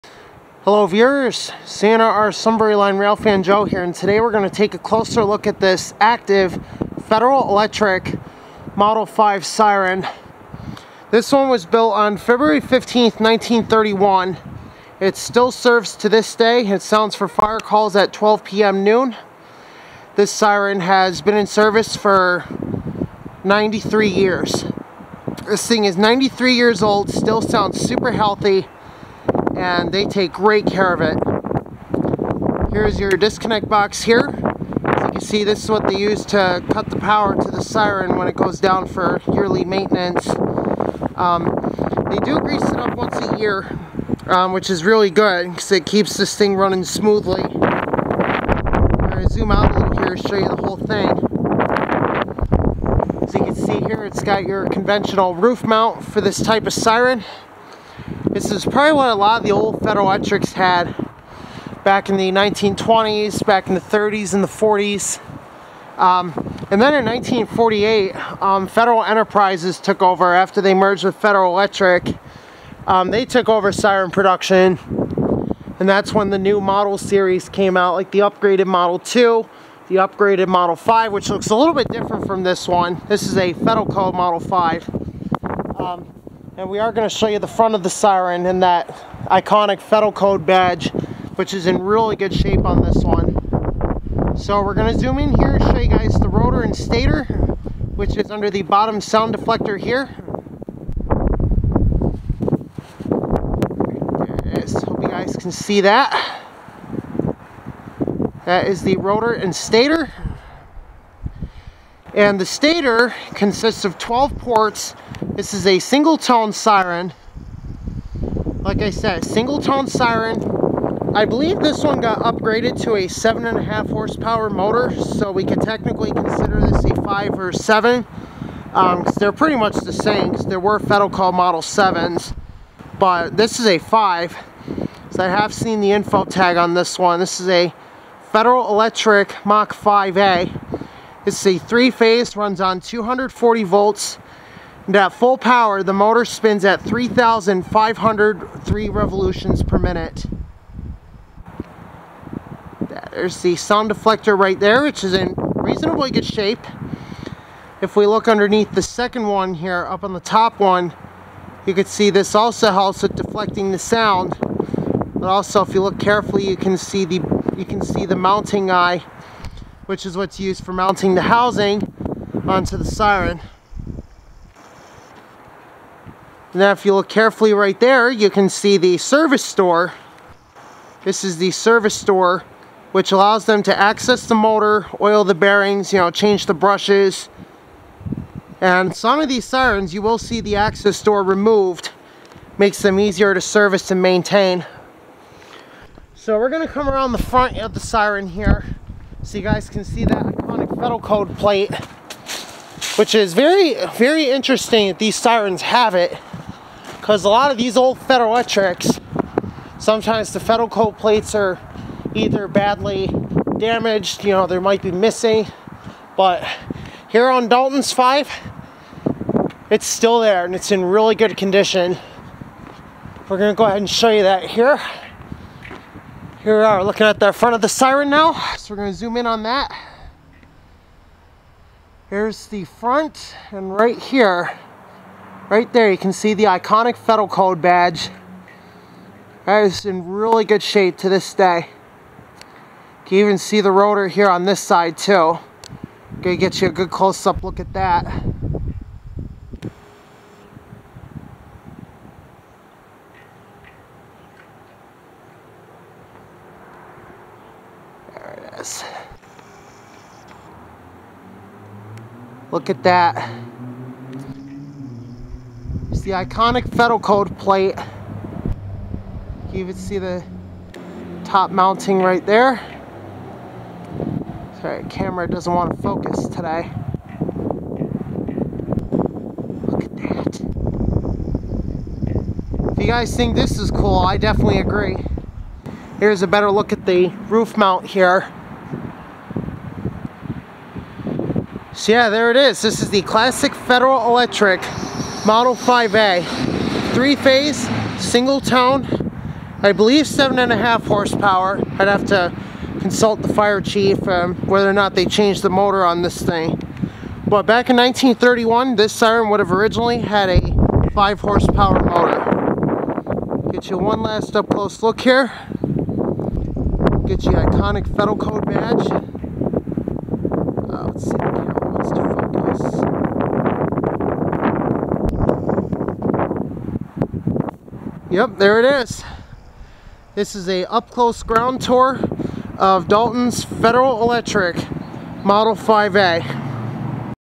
FEDELCODE Model 5A Siren Ground Tour PART 2 | Close Up Recording
A Much Better Close Up Video of an Active and VERY HEALTHY Fedelcode Model 5A..

Siren Information:

Siren: Federal Electric Mach 5A (Upgraded to 7.5 HP In 1967) Originally had a 5 HP Motor
Town: Dalton, Pennsylvania (Lackawanna County)
System: No System For This Siren
Location: Dalton Fire Company Station 5
Tested: 12 PM Monday-Saturday
Uses: Noon Blast Lunch Tradition and 3 Cycle Attack On Special Occasions
Cycle: STANDARD NOON BLAST (15 Second Test Signal)
Maintenance: As Needed With Yearly Greasing Of Motor Bearings

Other Notes:
Siren was built on Feb 15 1931 And sounds very healthy for a 93 year old siren. It was used as an air raid siren during WW2 and then as a civil defense and fire siren until being phased out by pagers and now only does a noon blast Monday through Saturday. It Will sound a 3 cycle attack signal on special occasions such as a test after maintenance or the lighting of the Christmas tree in December. Other than that it only sounds at noon Monday-Saturday.